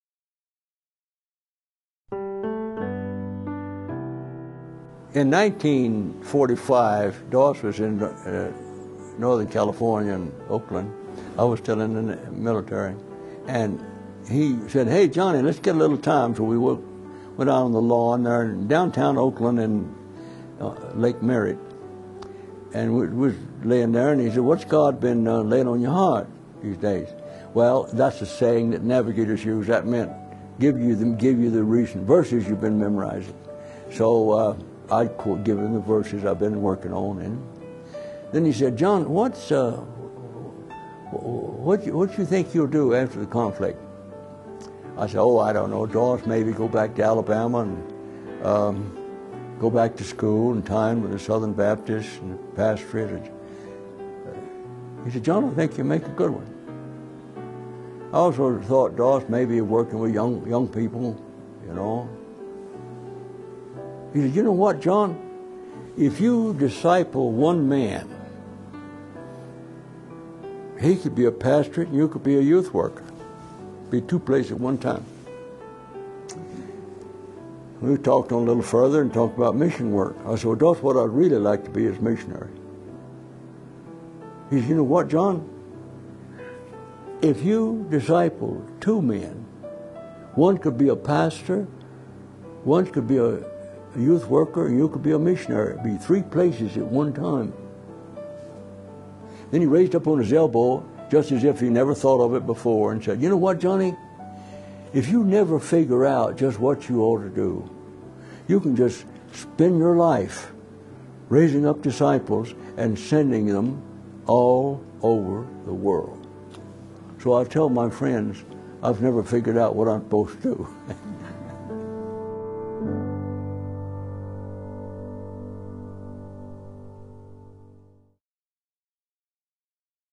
Discipleship Explained by Dawson Trotman - John Crawford
John Crawford sharing about the Founder of the Navigators and how God used a man like Dawson Trotman to start one of the largest discipleship ministries in the world.